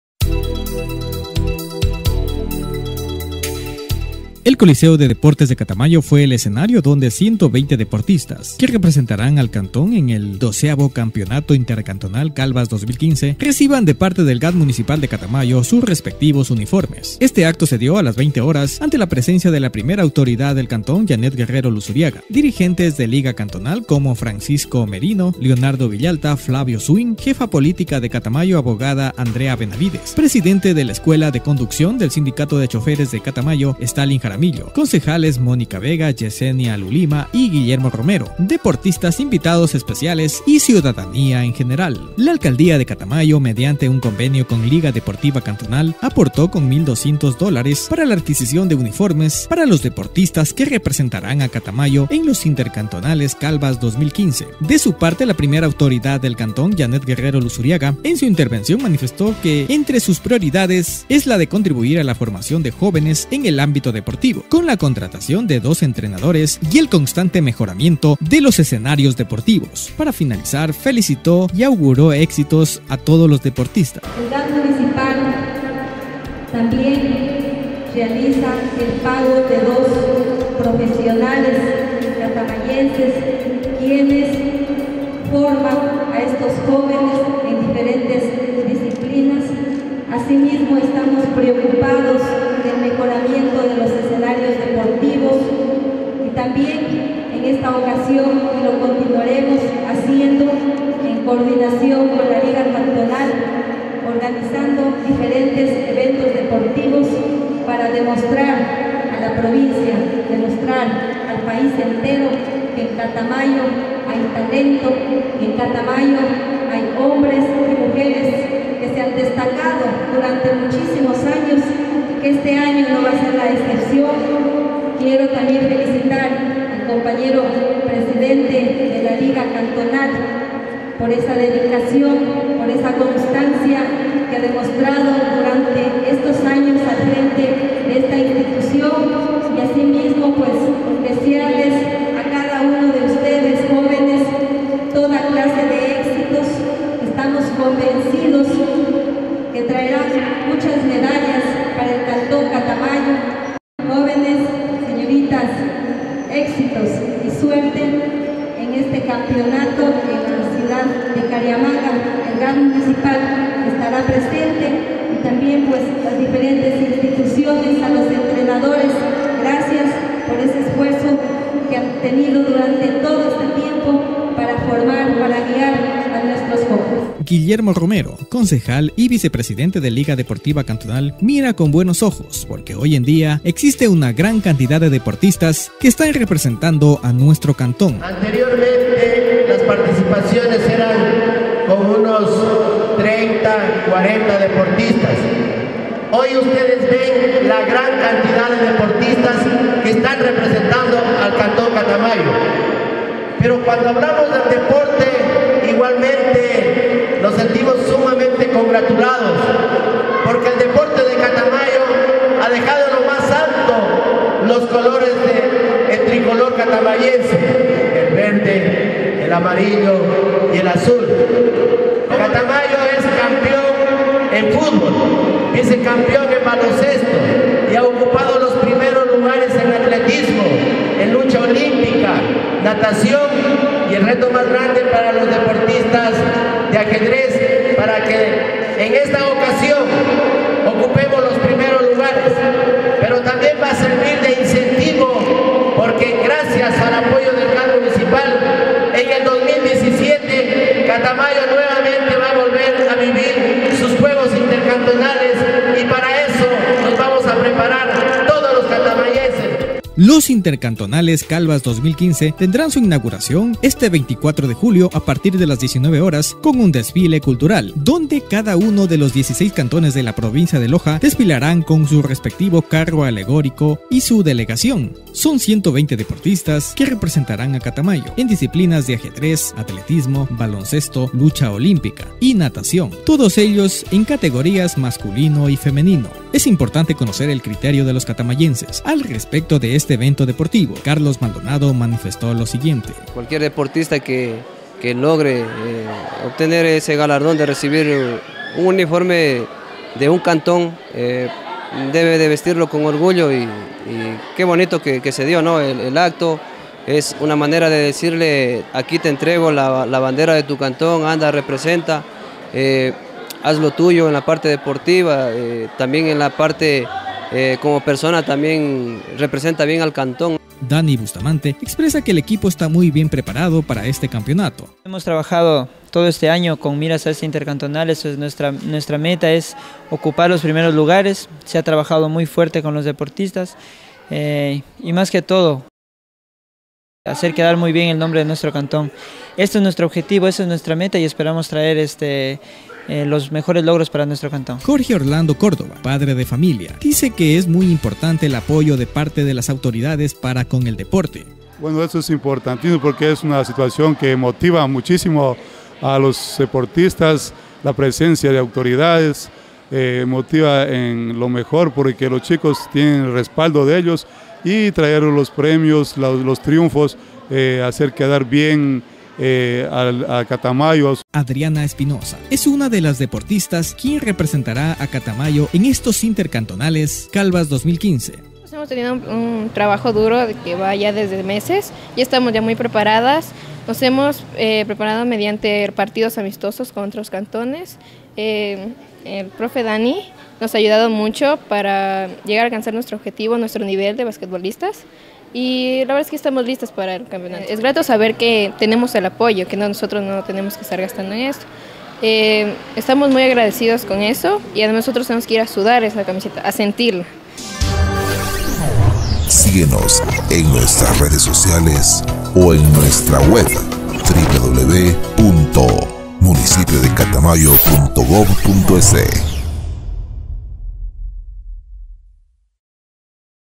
Catamayo.-│120 jóvenes que participaran en XII Campeonato Intercantonal Calvas 2015
Los Juegos Intercantonales tendrán su inauguración este  24 de julio a partir de las 19h00 con un desfile cultural, donde cada uno de los 16 cantones de Loja, desfilaran con su respectivo carro alegórico y su delegación.
Son 120 deportistas que representaran a Catamayo en disciplinas de Ajedrez, Atletismo, Baloncesto, Futbol, Lucha olímpica, Natación todos ellos en categorías masculino y femenino.